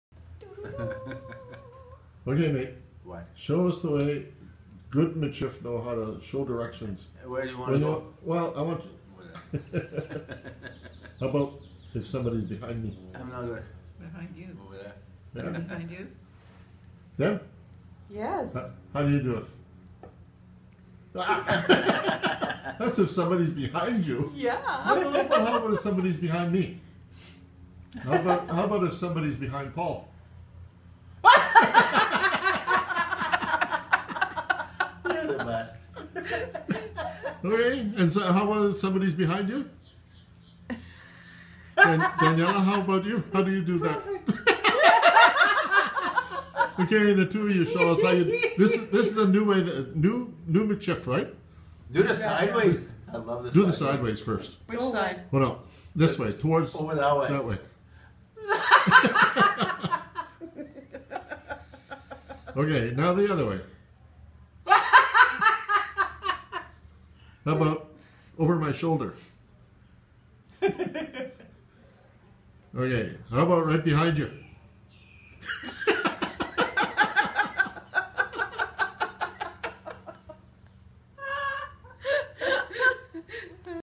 New Dynamic in Metis Culture
Paul, Deb & Danielle show the traditional Metis lip pointing method of giving directions...but with a twist ... Deb & Danielle cane up with a new way of pointing behind them!